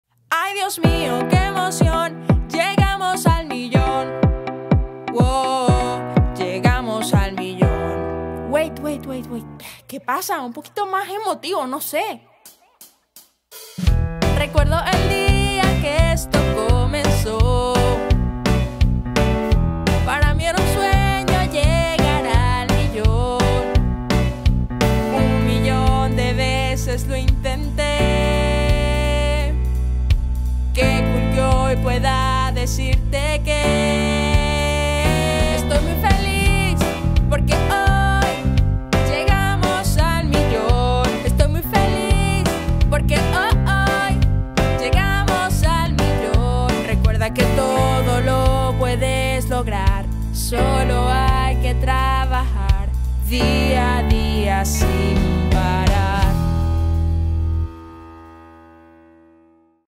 SOMOS UN MILLÓN ❤️😰 Janinalchair
Ay Dios mío que emoción LLEGAMOS AL MILLÓN ❤️❤️ gracias por su apoyo lo hice con mucho cariño, recuerda que todo lo puedes lograr solo hay que trabajar día a día sin parar